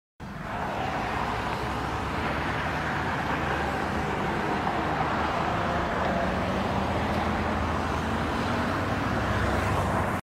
Corvette C8 prototype spotted in Colorado
Corvette C8 prototype driving in Colorado on March 21, 2018.  Too bad the car is so far away, with too much other traffic, to hear the engine.